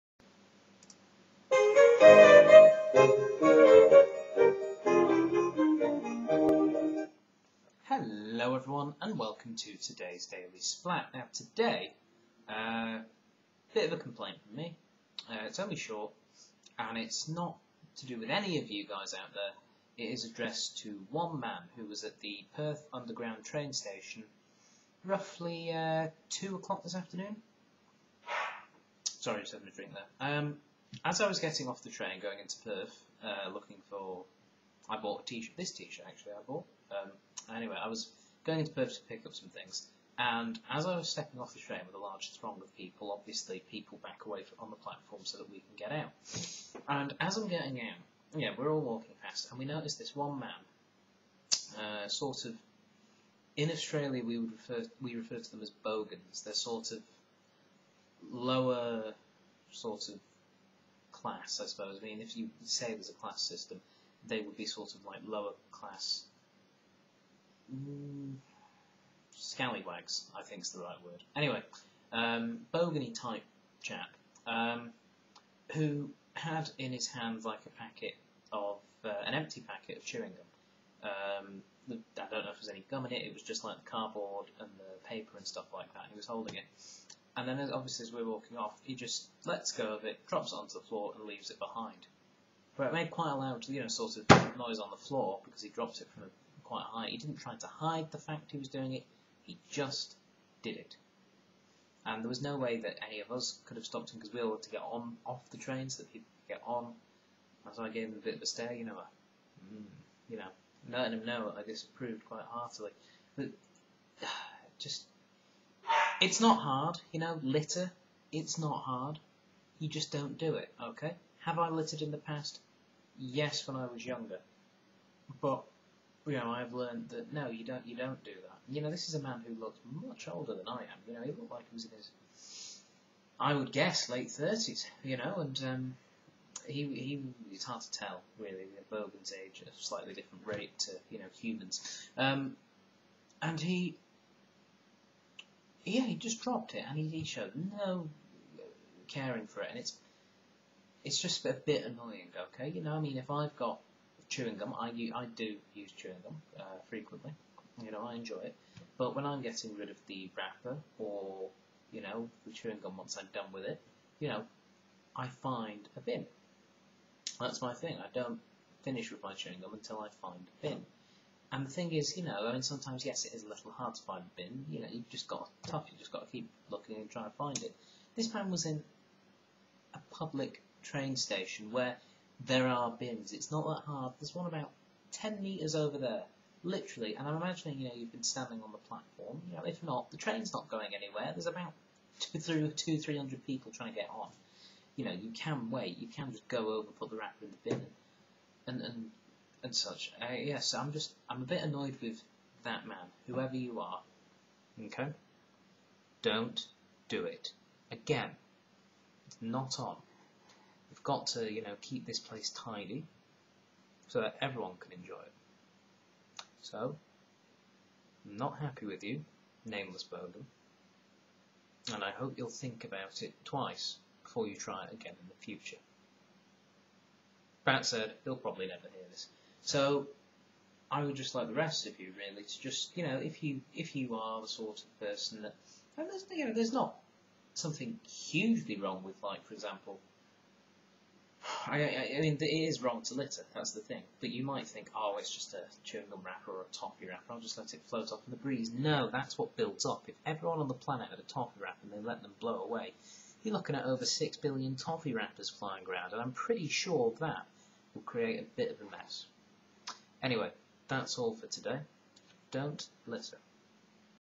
Daily Splatt - A Message for The Littering Bogan at the Perth Underground Train Station 9/3/2011
This video is for the Littering Bogan Splatt encountered today at Perth Underground Train Station.